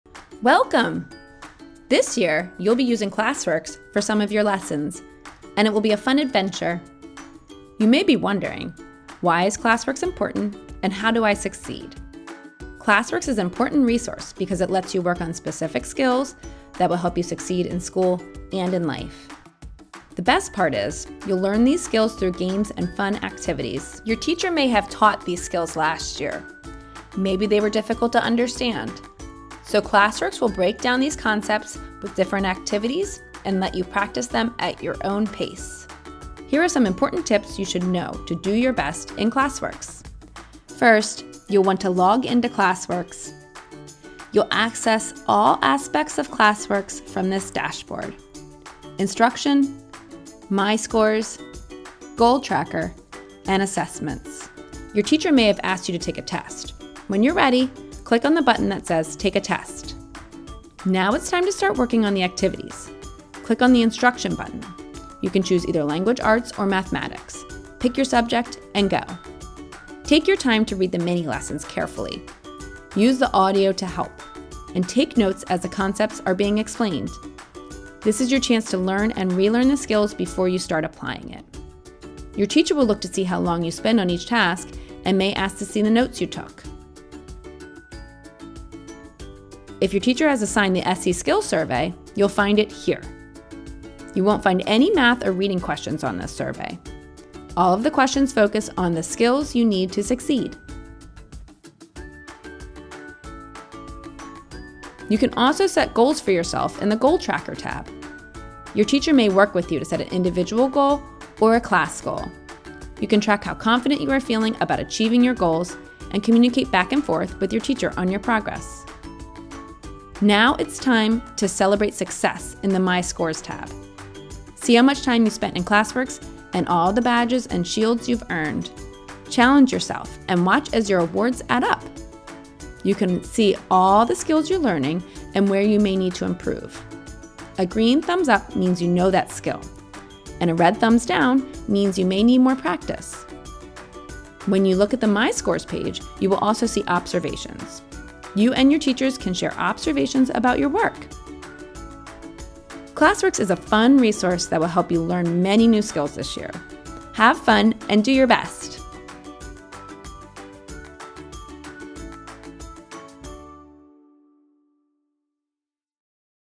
Why is Classworks Important for Students?
This year you’ll be using Classworks in your classroom.

Classworks is an important resource because it lets you work on specific skills that will help you succeed in school and life. The best part is, you’ll learn the skills through games and fun activities.

Follow along as we give you tips to do your best in Classworks.

CONNECT WITH CLASSWORKS ON SOCIAL